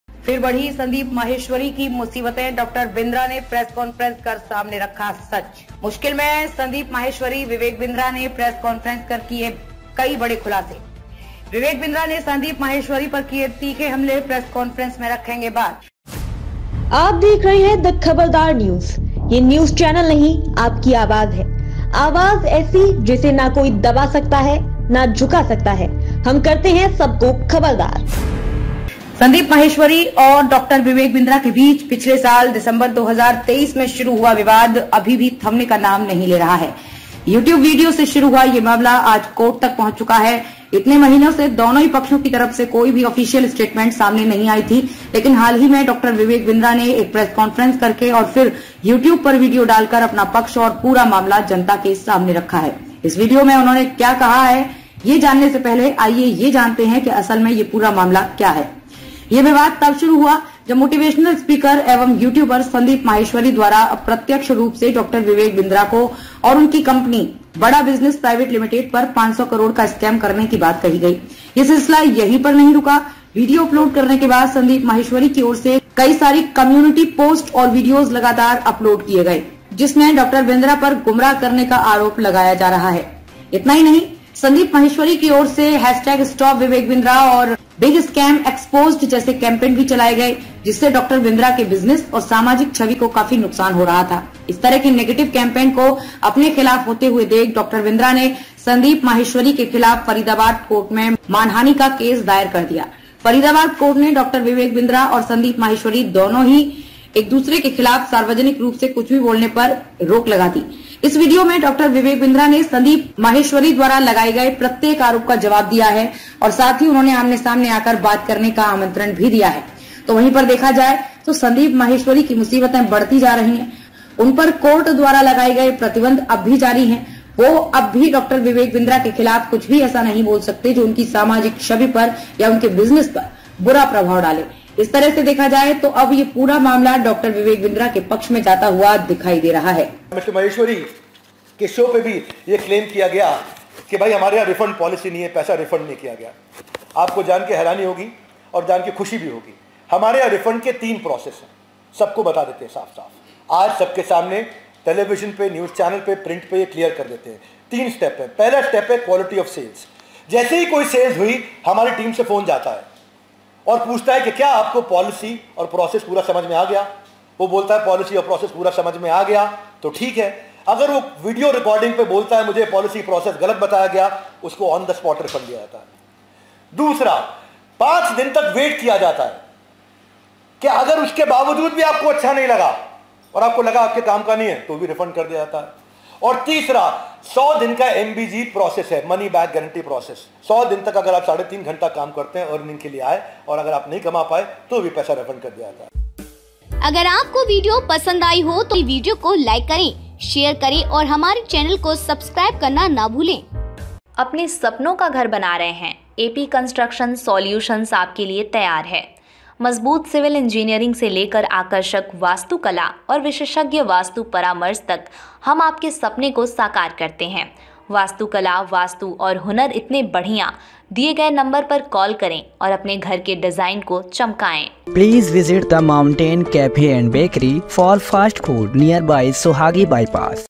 Vivek Bindra को मिला जनता का Support, Sandeep Maheshwari मामले में प्रेस कॉन्फ्रेंस कर पूरा मामला...
संदीप माहेश्वरी और  डॉ विवेक बिंद्रा के बीच पिछले साल दिसंबर 2023 में शुरू हुआ विवाद अभी भी   थमने का नाम नहीं ले रहा है। यूट्यूब वीडियो से शुरू ये मामला आज कोर्ट तक पहुंच चुका है। इतने महीनों से दोनों ही पक्षों की तरफ से कोई भी ऑफिशियल स्टेटमेंट सामने नहीं आई थी। लेकिन हाल ही में डॉ विवेक बिंद्रा ने एक प्रेस कॉन्फ्रेंस करके और फिर यूट्यूब पर वीडियो डालकर अपना पक्ष और पूरा मामला जनता के सामने रखा है । इस वीडियो में उन्होंने क्या कहा है ये जानने से पहले आईये ये जानते हैं कि असल में पूरा मामला क्या है?